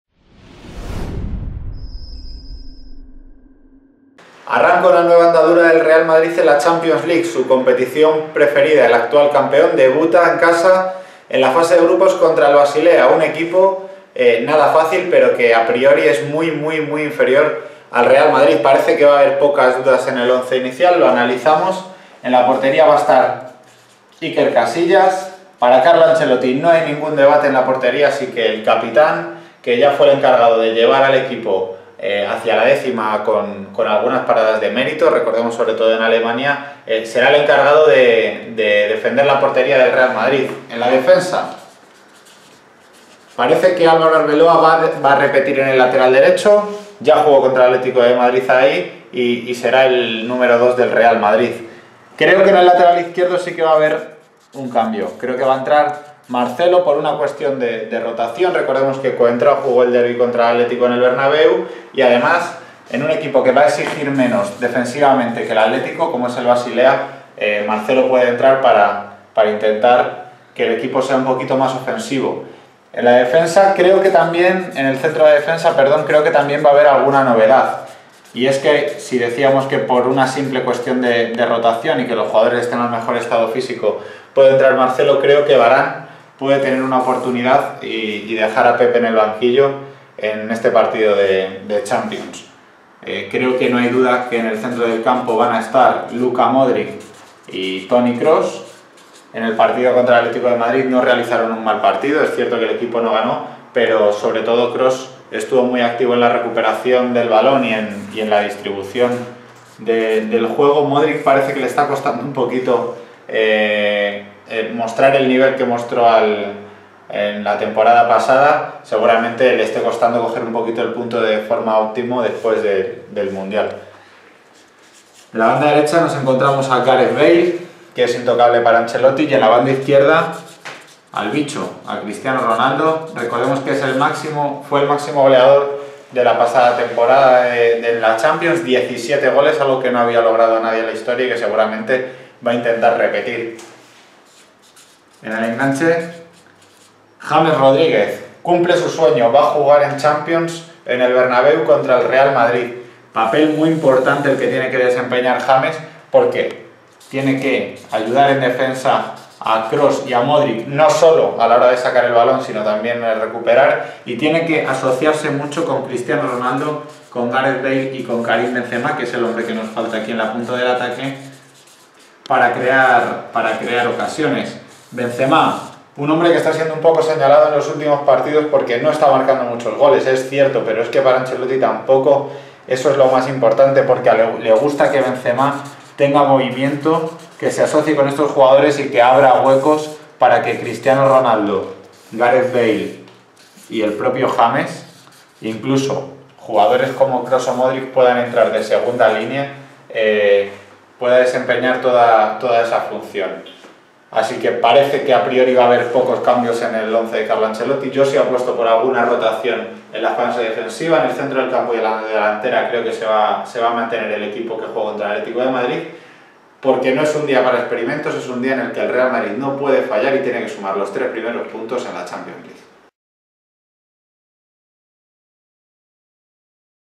Así jugará el Real Madrid contra el Basilea
Alineación del Real Madrid para el primer encuentro de la liga de campeones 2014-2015 que le medirá al Basilea.